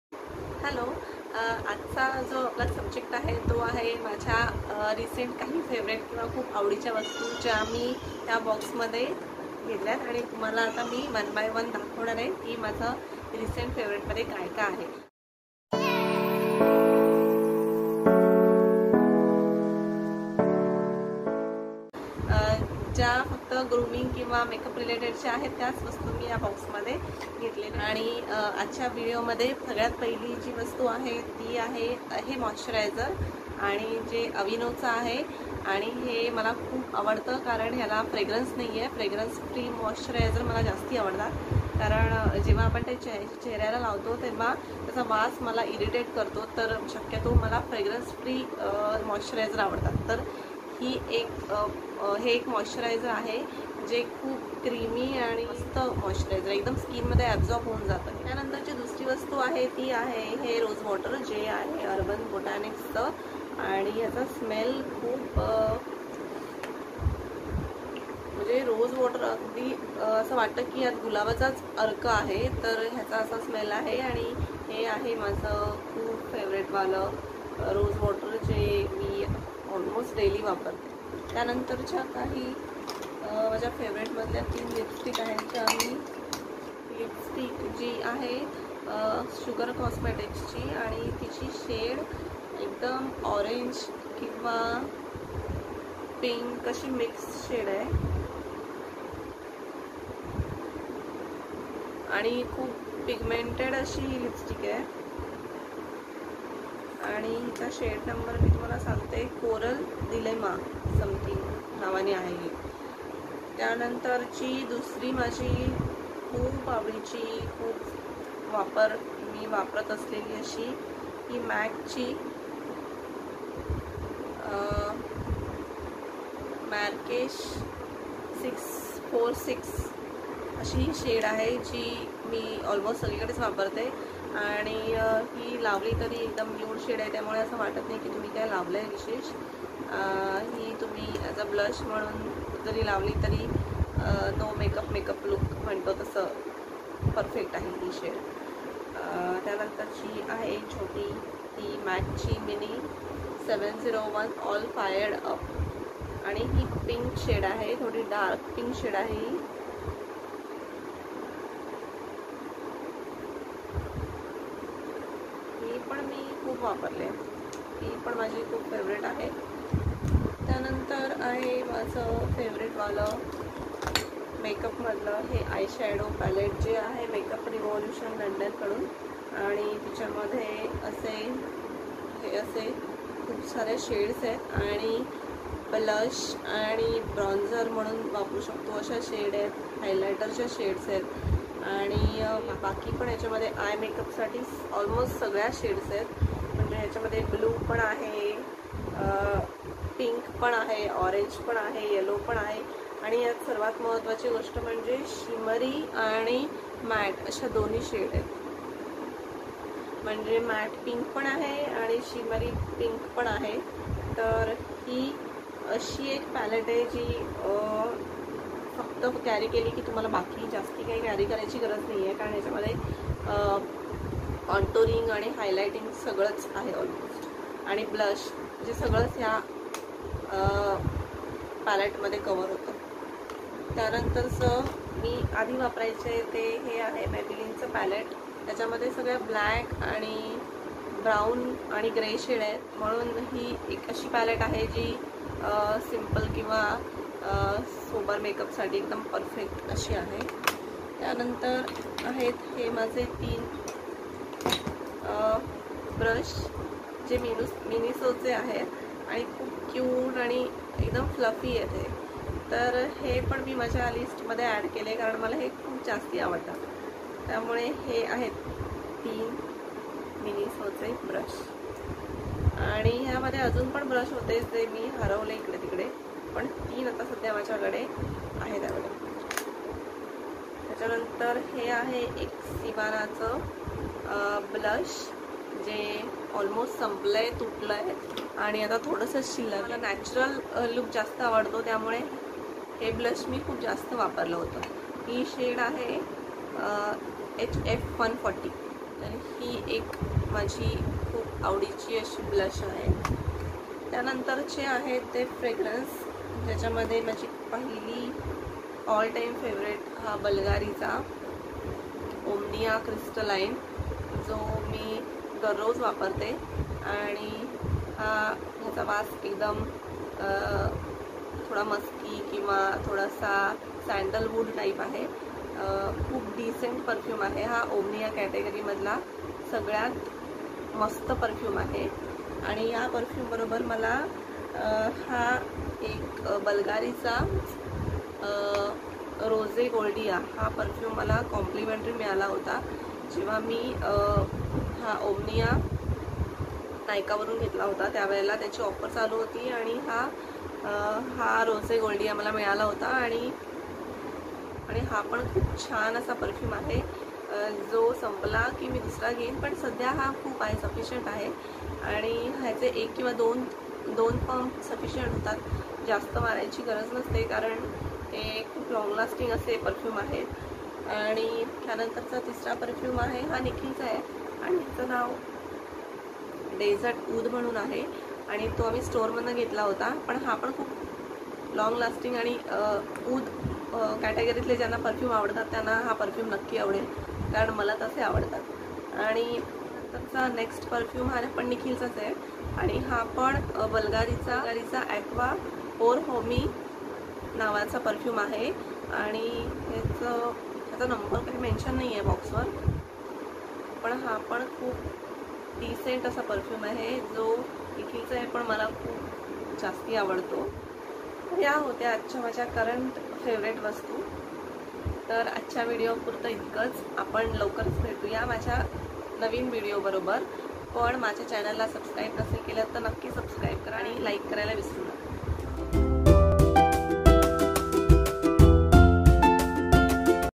Current obsessions | lipsticks💄| Fragrances| M.A.C.| Bvlgari Omnia |💛💚and much more with Pranoti
My todays products

👉🏼Aveeno daily moisturizer

👉🏼Urban botanics rose water

👉🏼 Sivaana blush

👉🏼Miniso makeup brush

👉🏼Makeup revolution eyeshadow palette